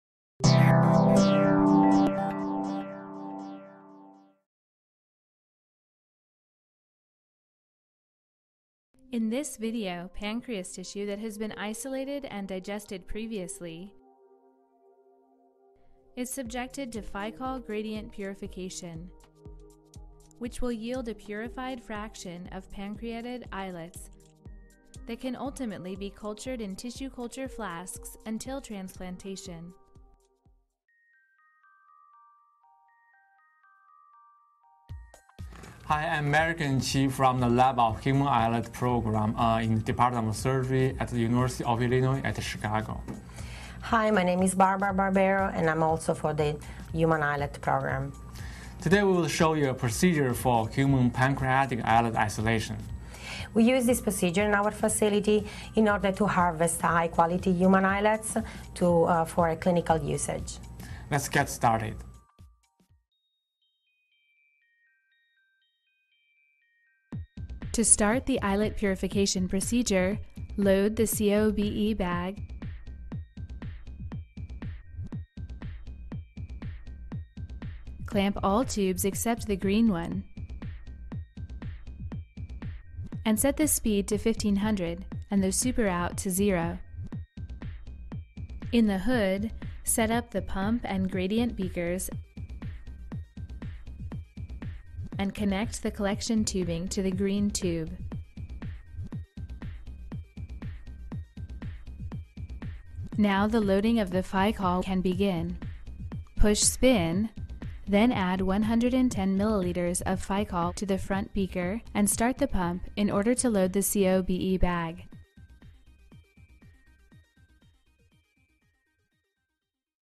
Human Pancreatic Islet Isolation: Purification & Culture-Human Islets l Protocol Preview
Human Pancreatic Islet Isolation: Part II: Purification and Culture of Human Islets -  a 2 minute Preview of the Experimental Protocol

Meirigeng Qi, Barbara Barbaro, Shusen Wang, Yong Wang, Mike Hansen, Jose Oberholzer
University of Illinois, Chicago, Department of Surgery;

Achieving high quality and appropriate quantity of human islets is one of the prominent prerequisites for successful islet transplantation. In this video, we describe step by step the procedures for human pancreatic islet isolation (part II: purification and culture of human islets) using a modified automated method.